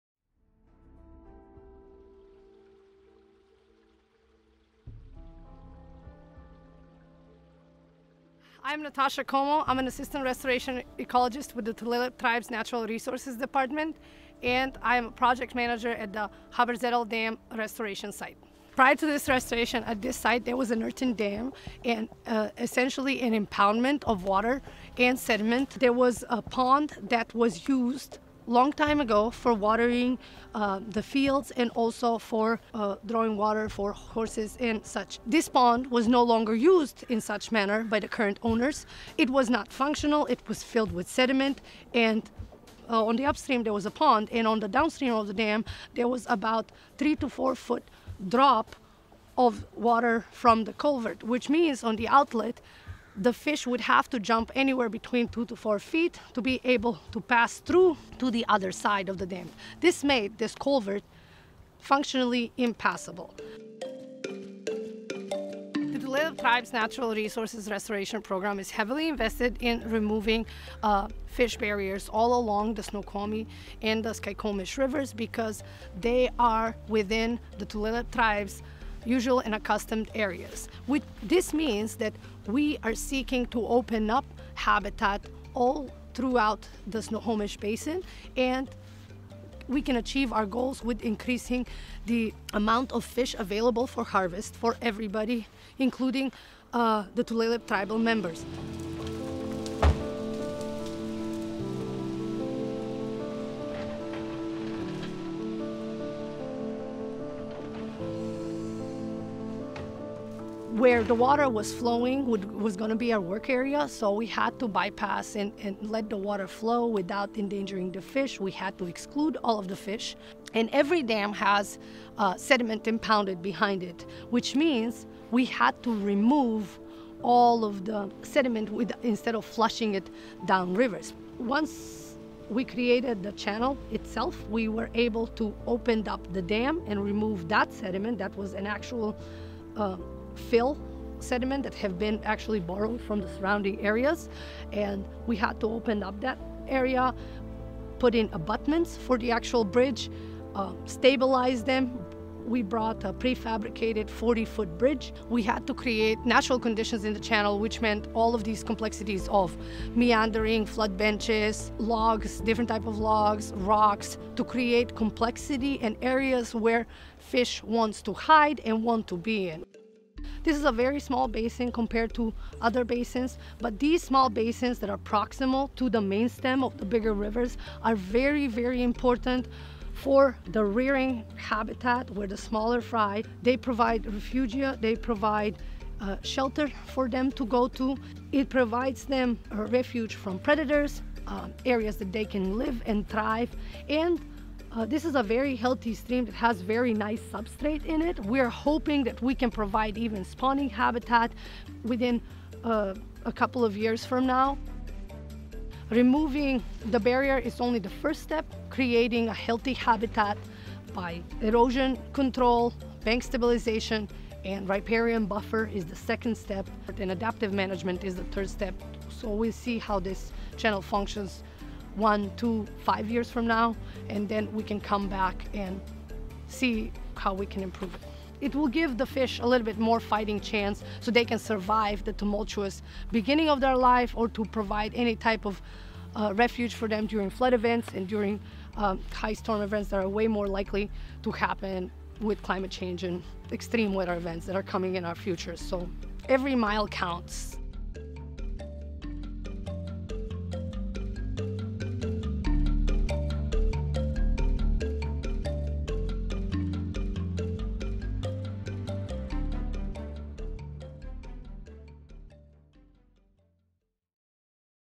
Every Mile Counts: The Haberzetle Dam Removal
The Tulalip Tribe Natural Resources Department leads the way  in a fish restoration project in Carnation, WA. Natasha Coumou Assistant Restoration Ecologist explains why.